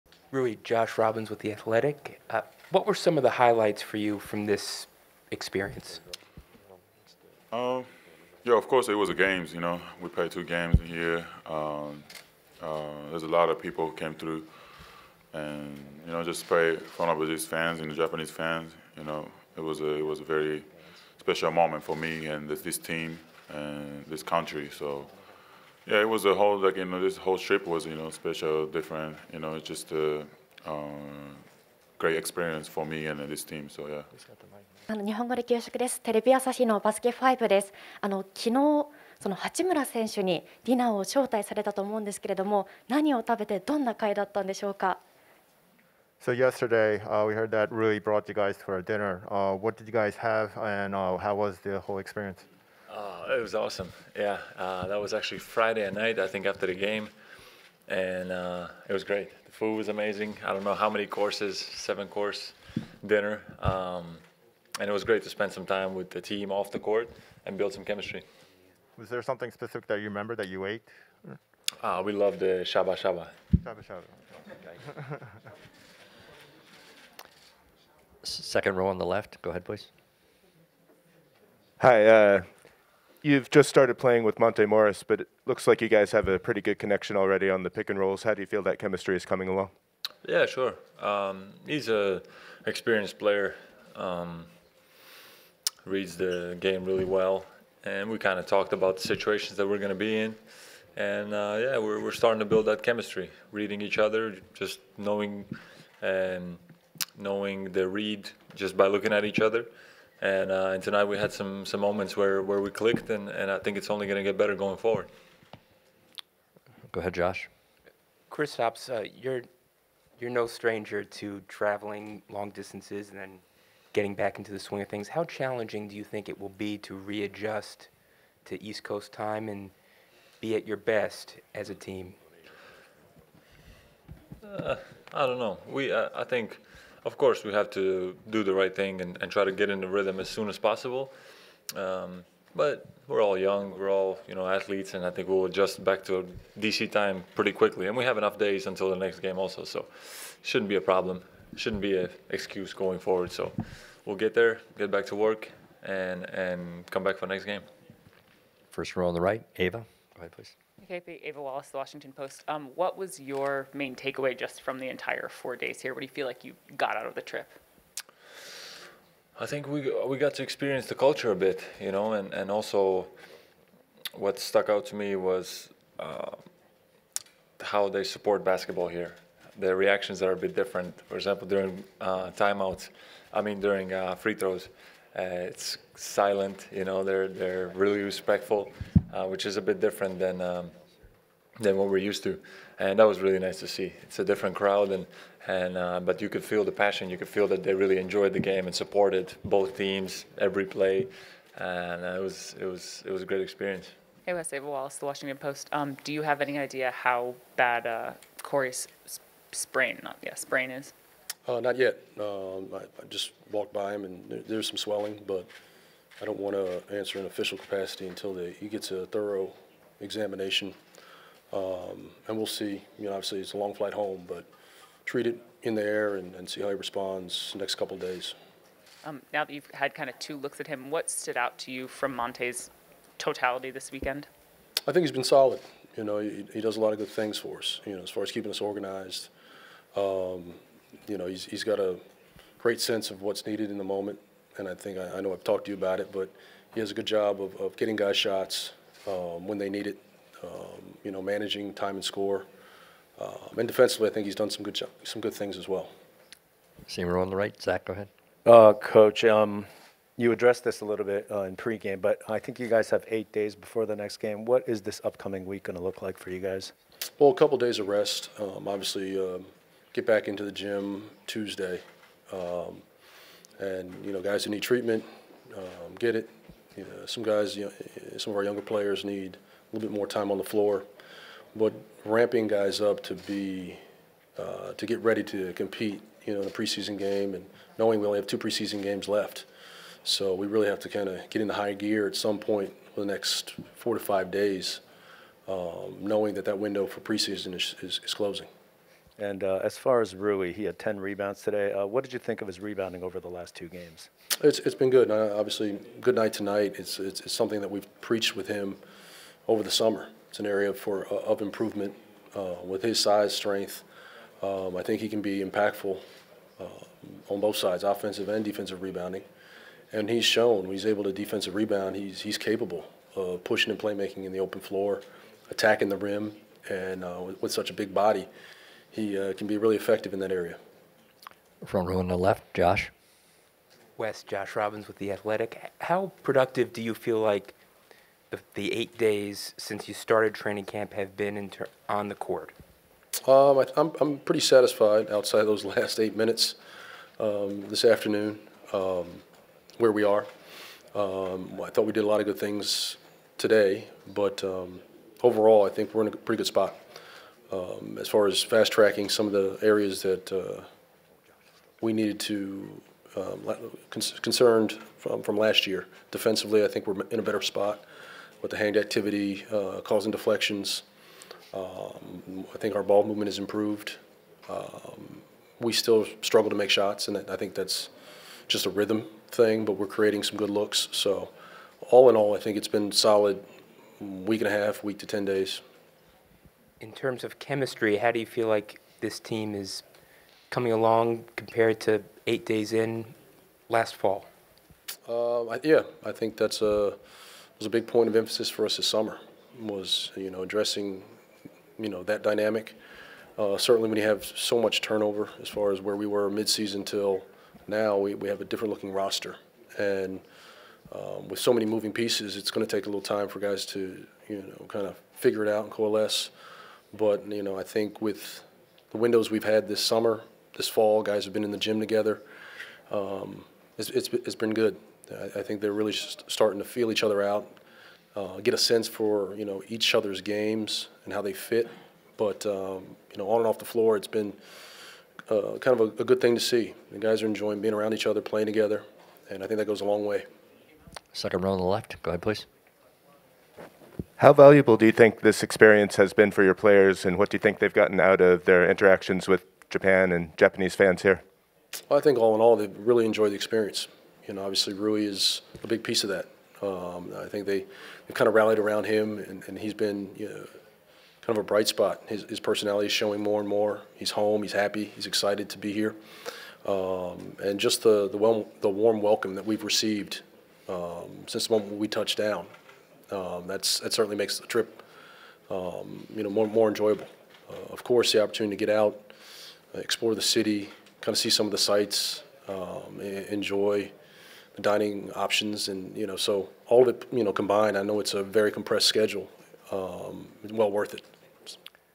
Postgame Press: WAS vs. GSW - Rui Hachimura, Kristaps Pozingis and Wes Unseld Jr. (September 2)
Rui Hachimura, Kristaps Pozingis and Wes Unseld Jr. met with the media after the Wizards’ preseason game against the Warriors on October 2 in Saitama, Japan.